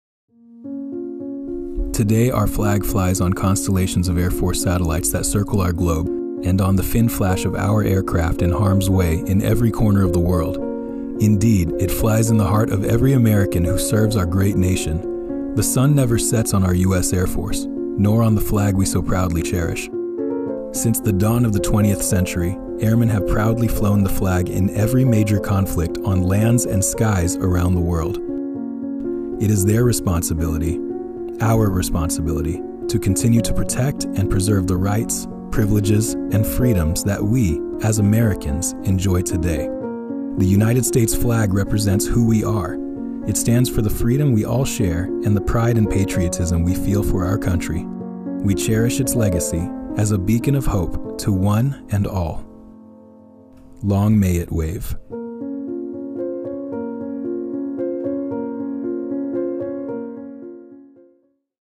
AFMAO commemorates Flag Day
Members of Air Force Mortuary Affairs Operations demonstrate, how to fold an American flag June 8, 2023.

DOVER AIR FORCE BASE, DE, UNITED STATES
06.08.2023

The United States officially adopted the flag on June 14, 1777. (U.S. Air Force video by Staff Sergeant Andrew J. Alvarado and Jason Minto)

Film Credits: Video by Staff Sgt. Andrew Alvarado and Jason Minto
Air Force Mortuary Affairs Operations